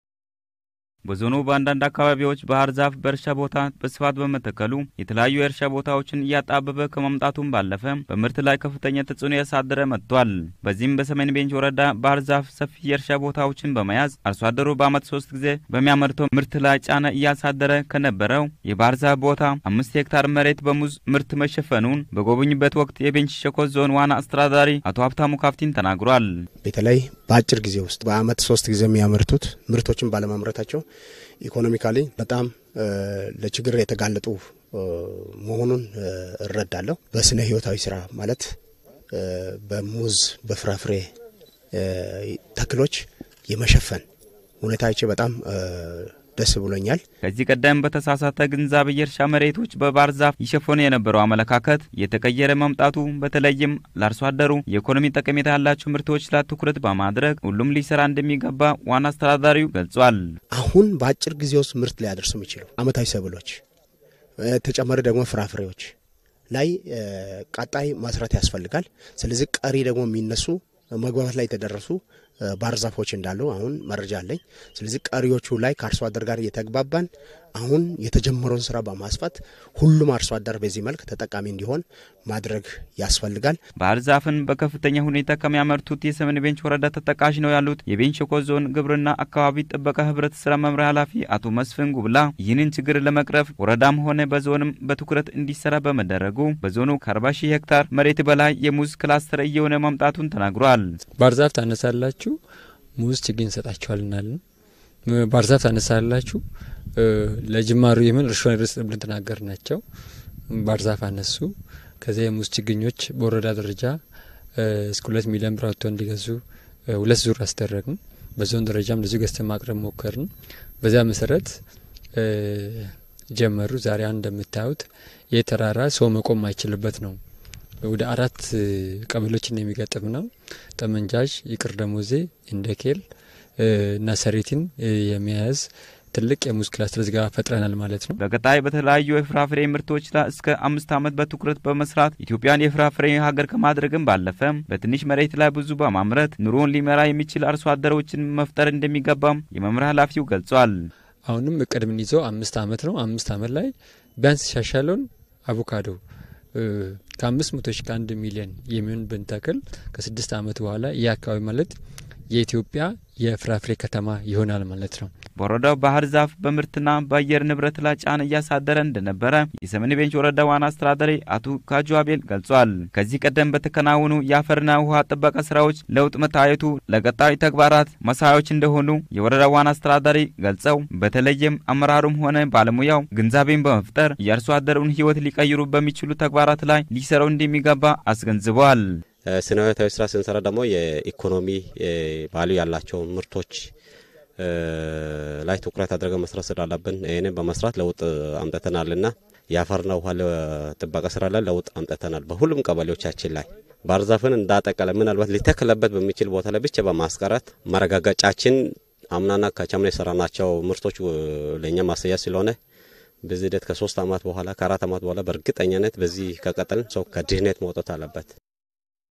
በተቀናጀ የተፋሰስ ልማት ሥራ ለአርሶ አደሩ የኢኮኖሚ ጠቀሜታ ያላቸው ምርቶች ላይ ትኩረት ሰጥቶ መሥራት እንደሚገባ የቤንች ሸኮ ዞን አስተዳደር ገለጸ
ዘጋቢ፡ አብዲሳ ዮናስ- ከሚዛን ቅርንጫፍ